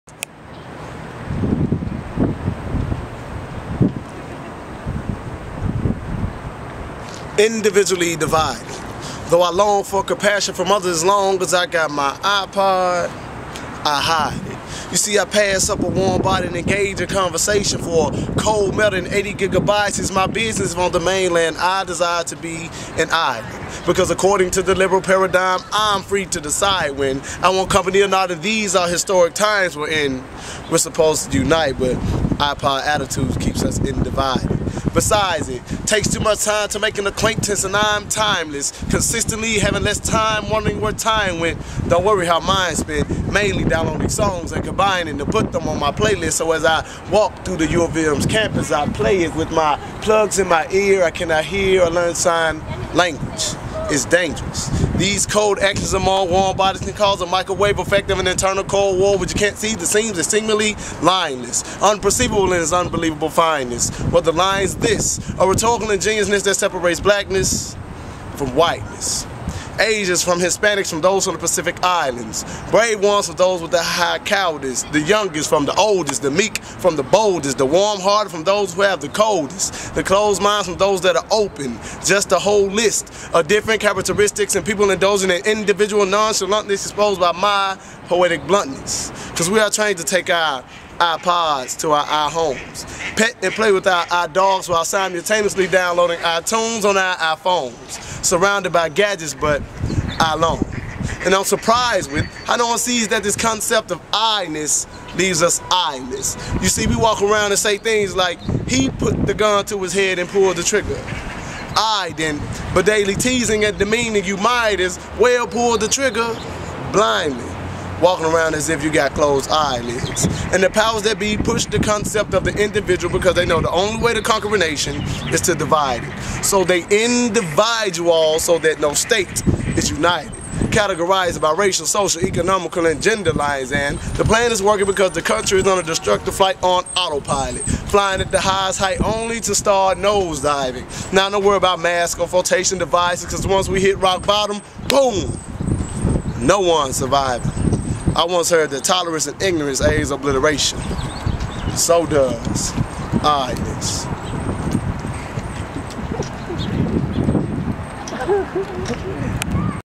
iPod World
This was shot in Nap town before a feature at the Earth House (shout out to Sleepy P the Copastetic). It was shot at the canal by AT Chill, the director. It served as a string of video shot that day of one of four poets, including me of course, but because I had not talked to the other poets I will not mention their names but check it out, it's a rare occurance.